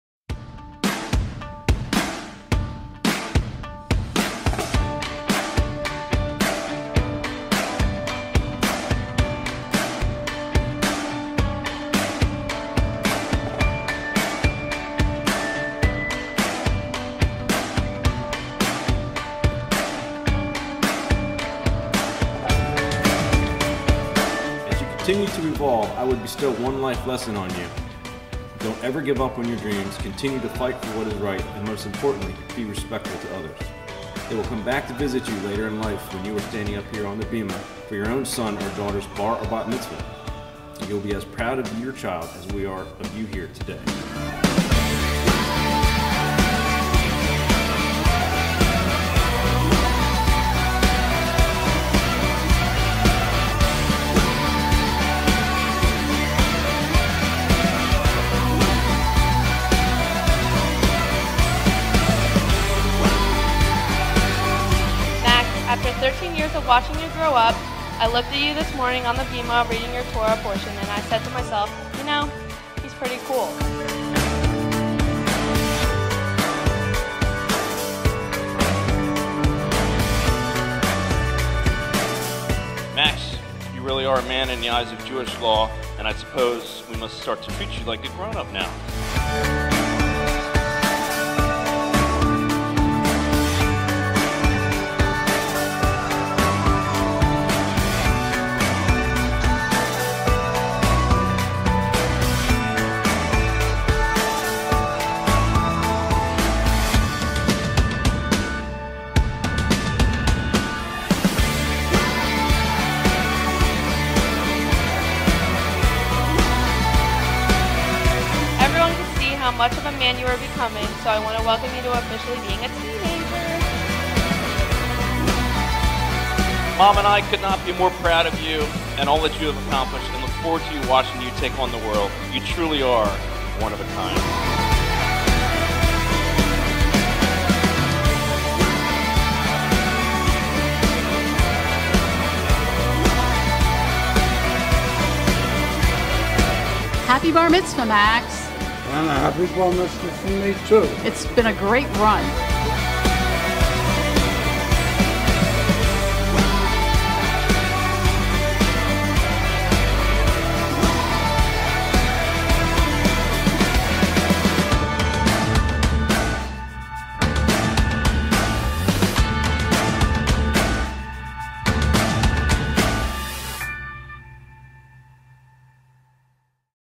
Max's Bar Mitzvah Trailer!
Max & Family had a blast with Hot Hot Hot Entertainment at the Sheraton Valley Forge!